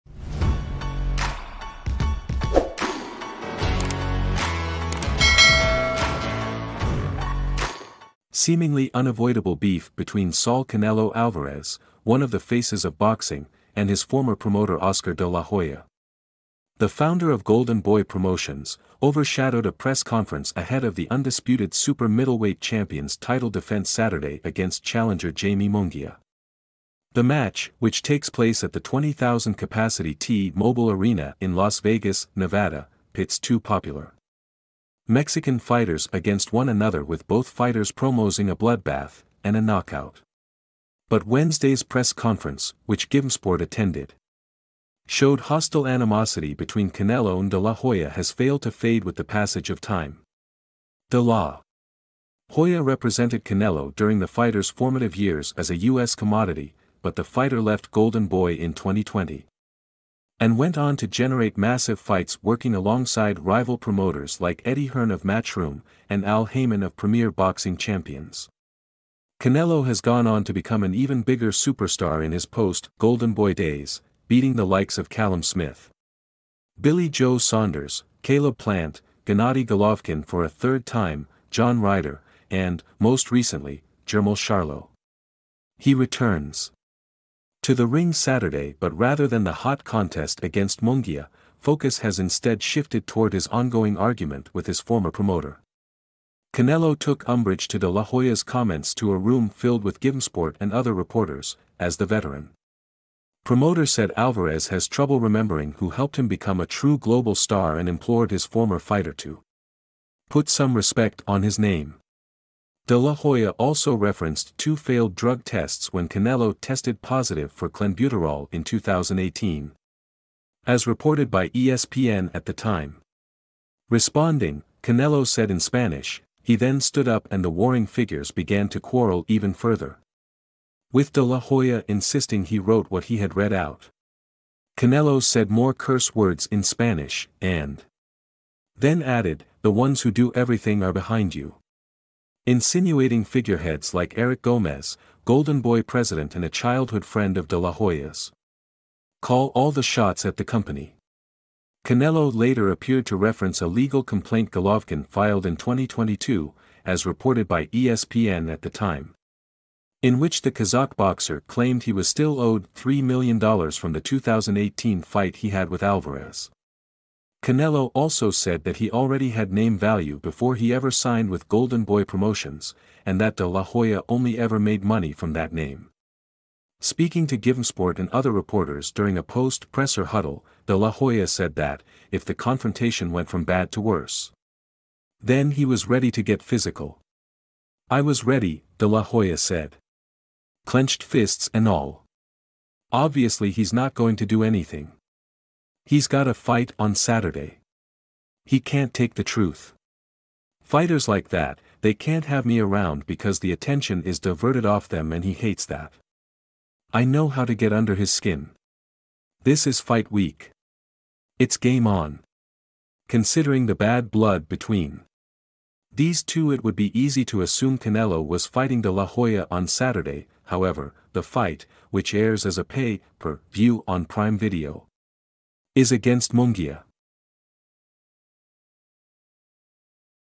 Canelo's Outburst at Oscar De La Hoya Translated to English - Boxing News
Seemingly unavoidable beef between Saul 'Canelo' Alvarez, one of the faces of boxing, and his former promoter Oscar de la Hoya, the founder of Golden Boy Promotions, overshadowed a press conference ahead of the undisputed super middleweight champion's title defense Saturday against challenger Jaime Munguia. The match, which takes place at the 20